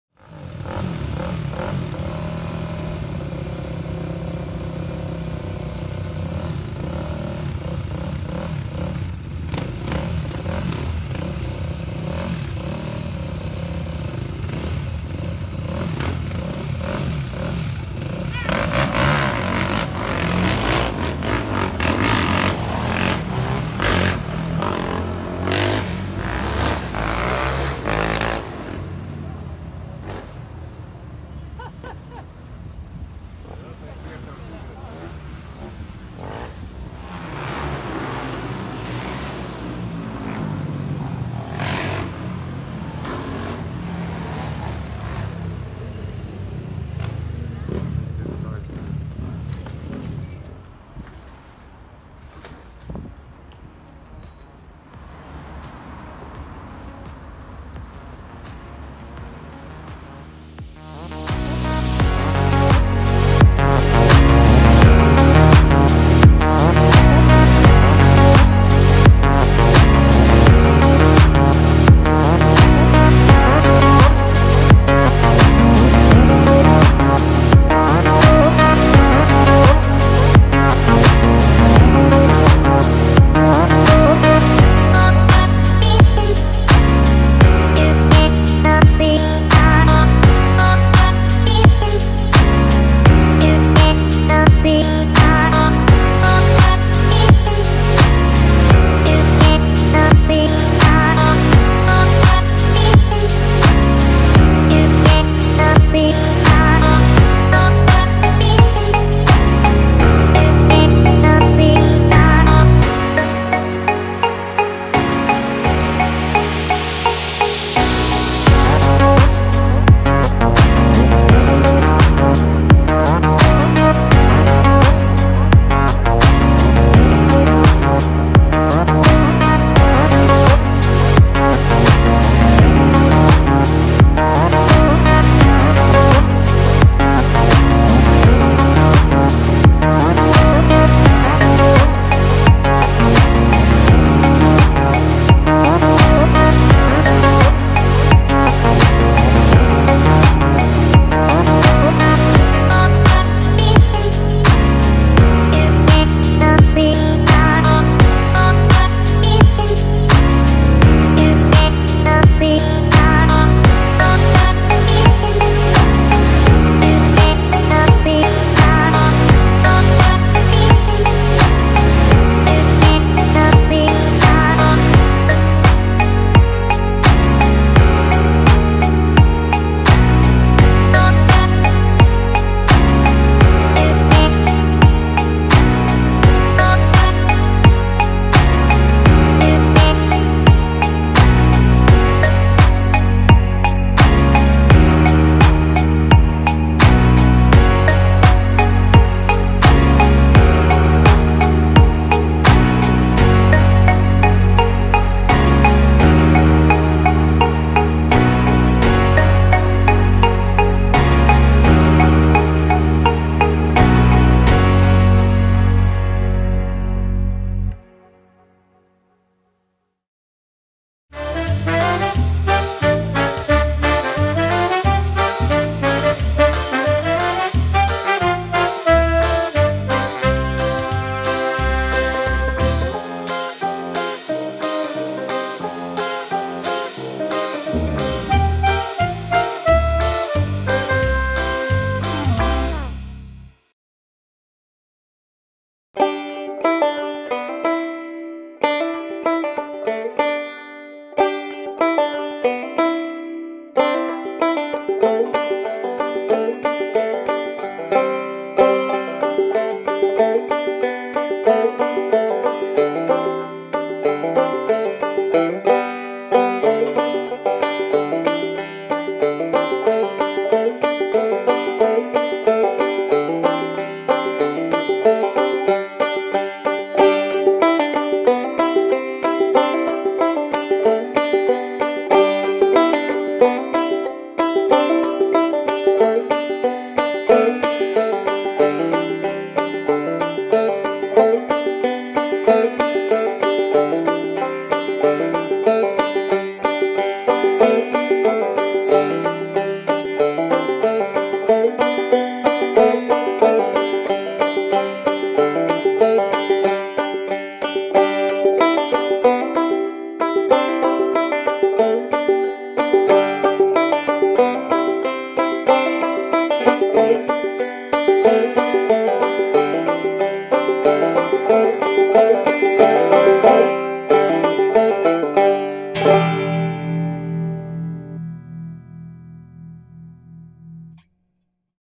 Barbados to Martinique - Ep.024
This week we sail from Barbados to Martinique and get our first impressions of beautiful Martinique
Music